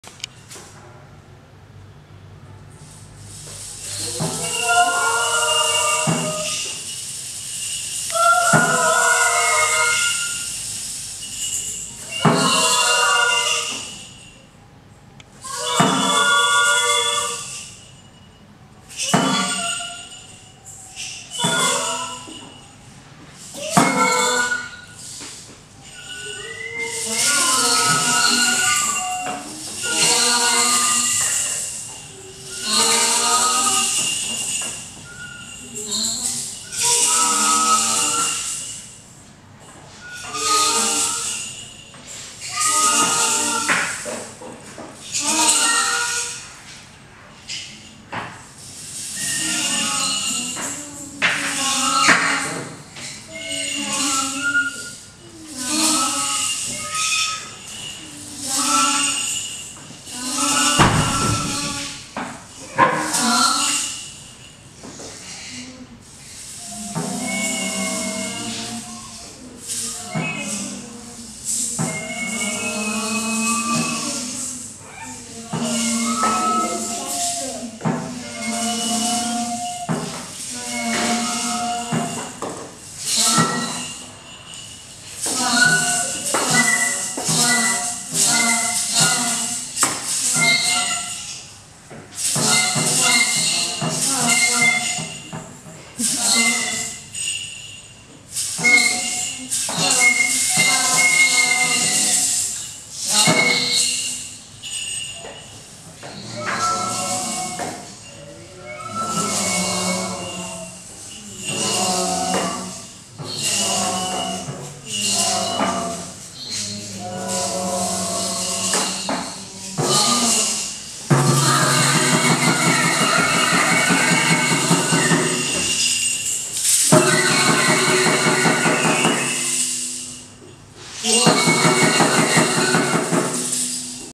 Make-it: Visual Score with Metta Theatre - 24th October - Performance 6
Families created and performed graphic scores of their own music composition, with composer Oliver Brignall as part of the V&A's Family Art Fun half-term programme. Find out more at vam.ac.uk/families